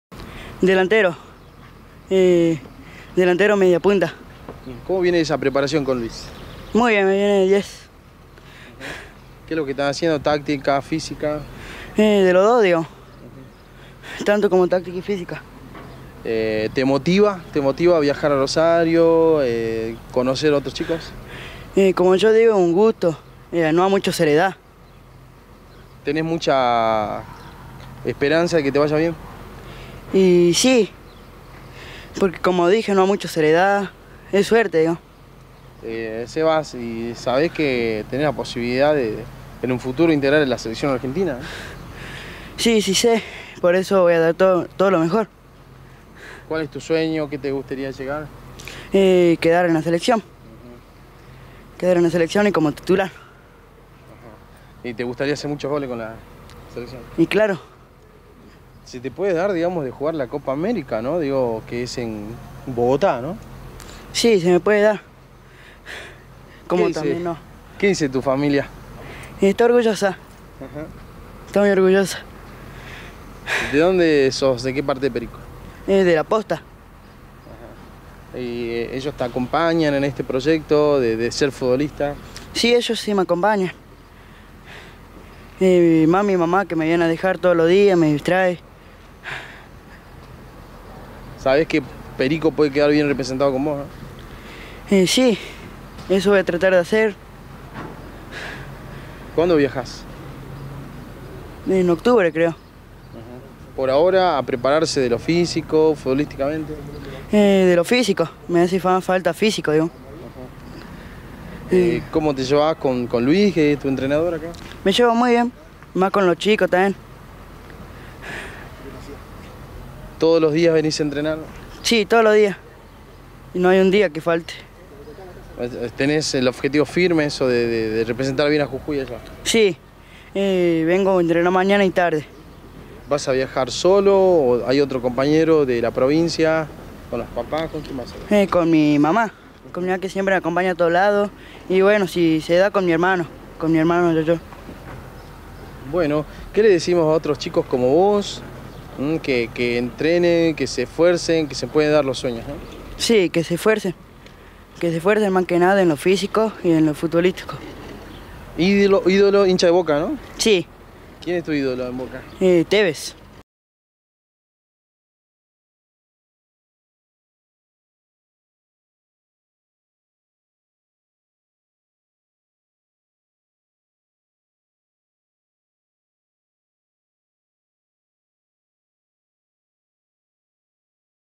SEBASTIAN VELASQUEZ LA JOVEN PROMESA DEL FUTBOL PC
PRUEBA EN ROSARIO PARA INTEGRAR LA SELECCIÓN JUVENIL DE FUTBOL PC.